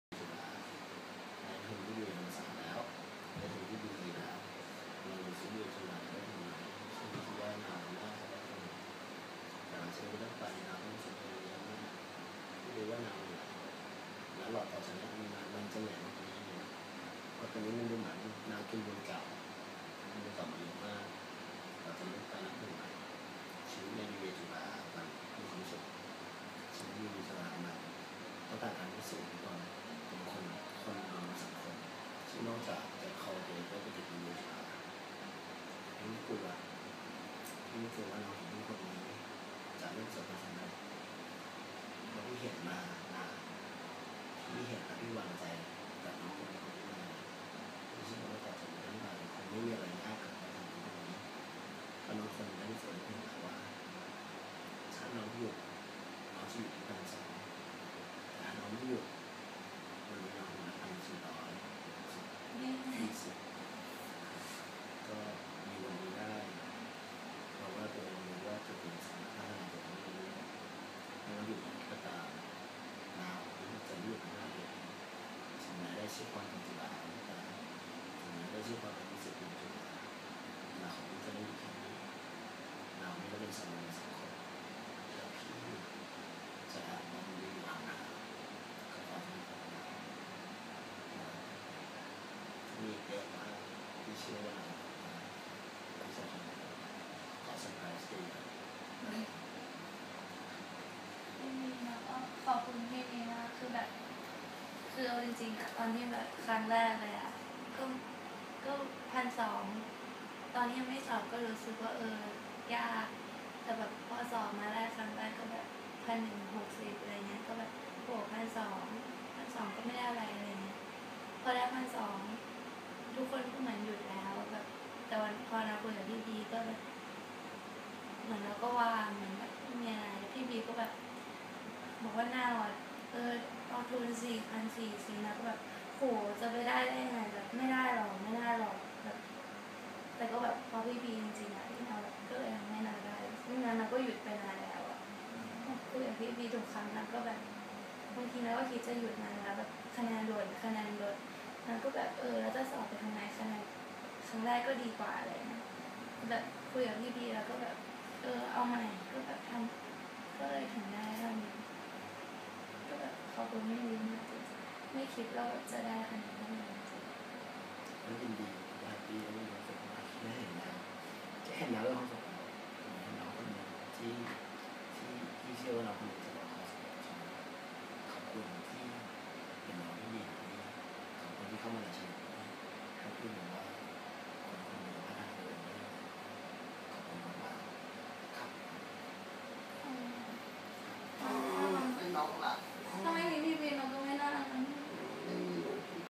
Congratulations Warwicksters 2012: Nong Now Triam Udom, BBA CU Early Application
Congratulations all Warwickster รุ่น 3!!!

We all make the history [:)]
There are (only the first three most competitive faculties announcement)
76 BBA THAMMASAT รุ่น 21
123 BE THAMMASAT รุ่น 20
24 BBA CHLUA รุ่น 17
41 EARLY APPLICATION BBA CHULA รุ่น 17

Finally we make it!